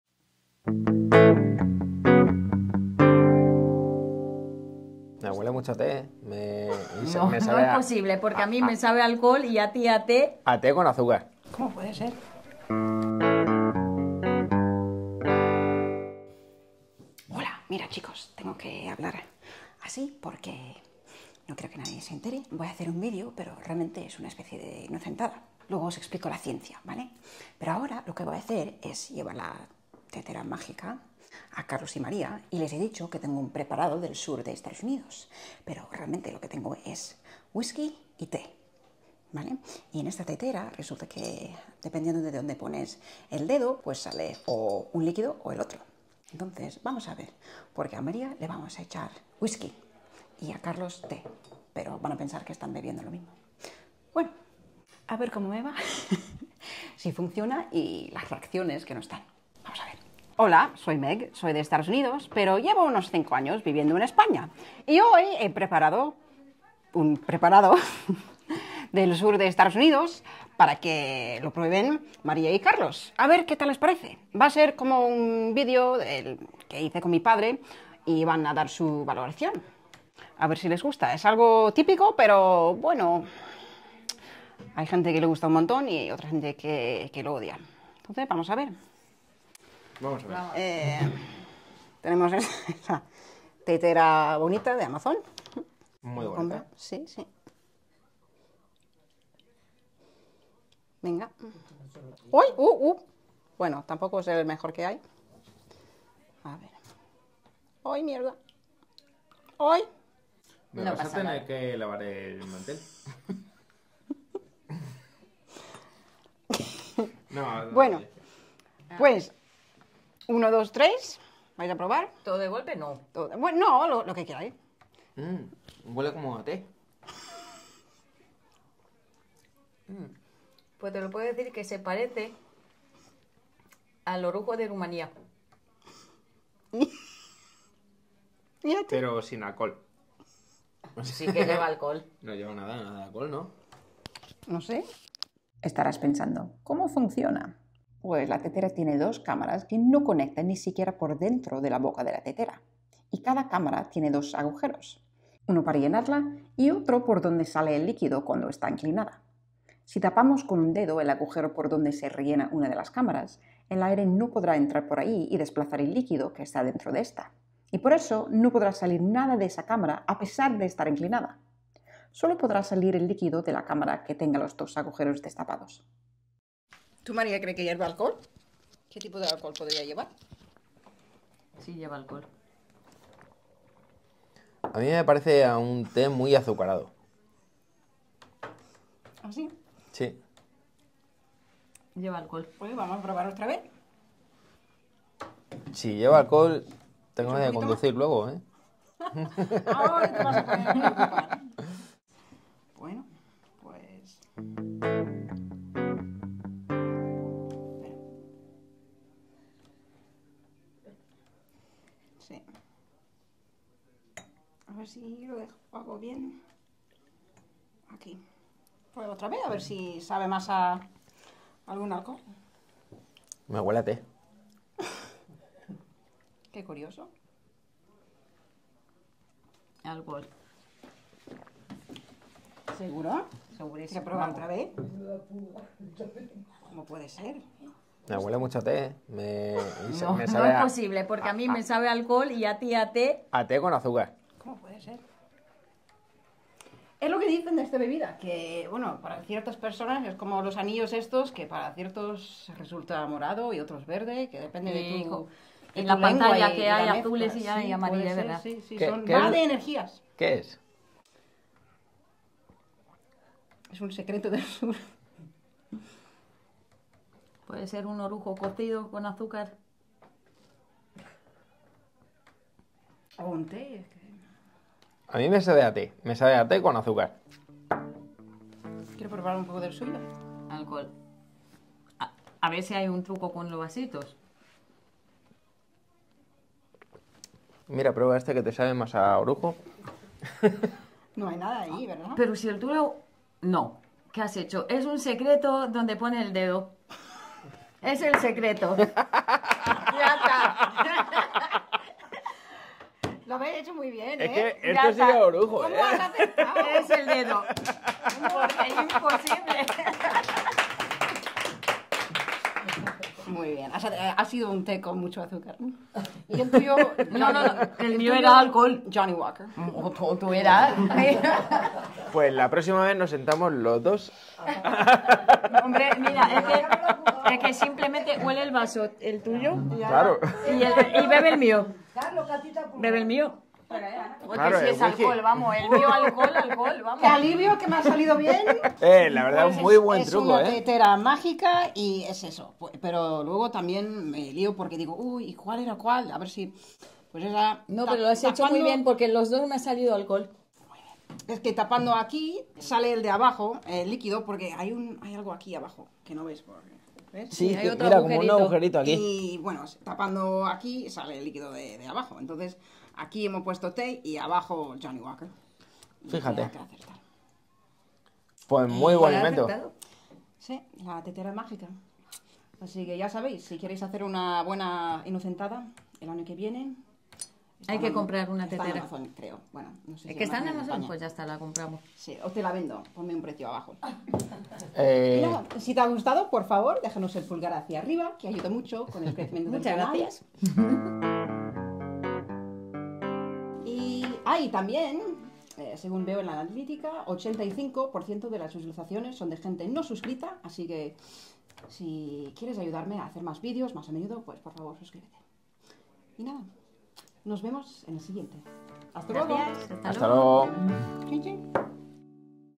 Mi inocentada con la “tetera de asesino” en España | My "Assassin's Teapot" Prank in Spain
Sorprendo a Carlos y Maria con un supuesto brebaje mágico utilizando la “tetera mágica” que compré por Amazon. ¿Averiguarán el truco?

Instagram: @MEGrisolano  Carlos y Maria de @eljardindebabia de Talavera de la Reina

I surprise Carlos and Maria with a supposedly magical concoction using the "magic teapot" I bought on Amazon. Will they discover the trick?

0:00 Intro
2:00 Primeras reacciones
2:25 La ciencia
2:56 Más reacciones
4:42 La hipótesis
5:51 Les explico
6:33 Conclusiones